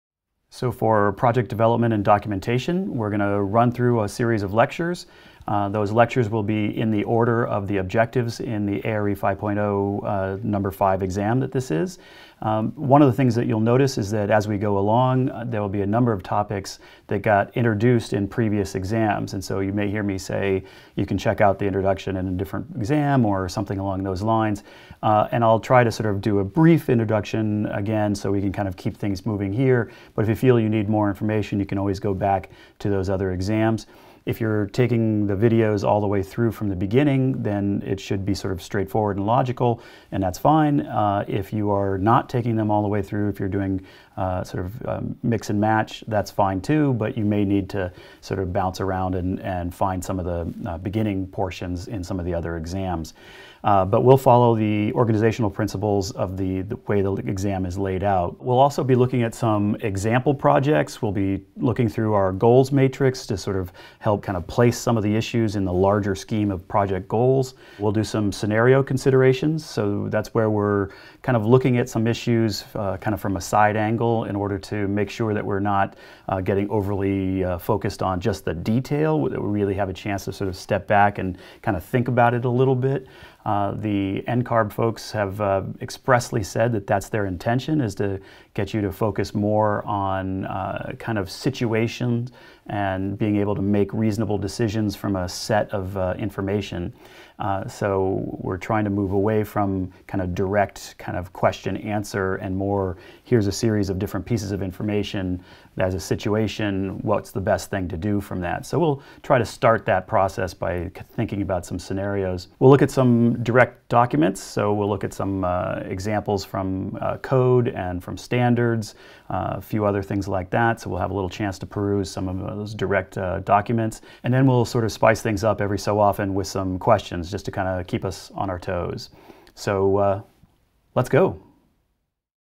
Project Development and Documentation Table of Contents
In this ARE 5.0 NCARB-approved Project Development and Documentation Exam Prep course you will learn about the topics covered in the ARE 5.0 PDD exam division. A complete and comprehensive curriculum, this course will touch on each of the NCARB objectives for the ARE 5.0 Project Development and Documentation Exam.

Instructor Mike Newman will discuss issues related to the development of design concepts, the evaluation of materials and technologies, selection of appropriate construction techniques, and appropriate construction documentation.

When you are done with this course, you will have a thorough understanding of the content covered in the ARE 5.0 Project Development and Documentation Exam including integration of civil, structural, mechanical, electrical, plumbing, and specialty systems into overall project design and documentation.